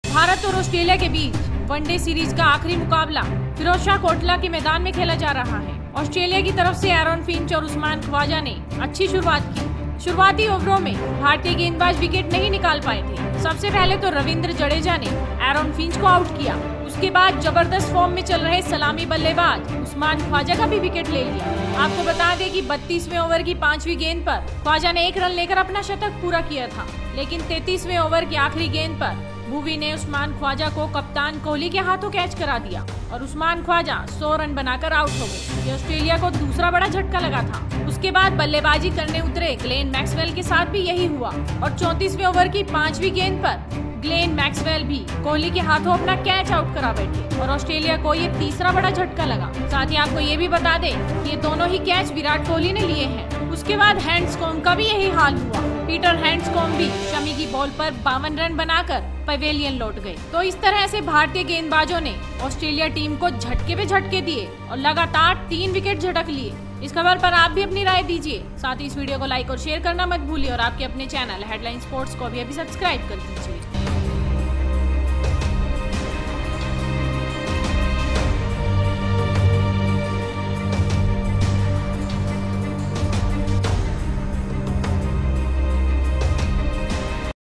Ind Vs Aus 5th ODI: भारतीय गेंदबाजों का चला जादू, ऑस्ट्रेलिया को लगे 3 बड़े झटके । Headlines Sports
Ind Vs Aus 5th ODI: भारतीय गेंदबाजों का चला जादू, ऑस्ट्रेलिया को लगे 3 बड़े झटके Must Watch This Full News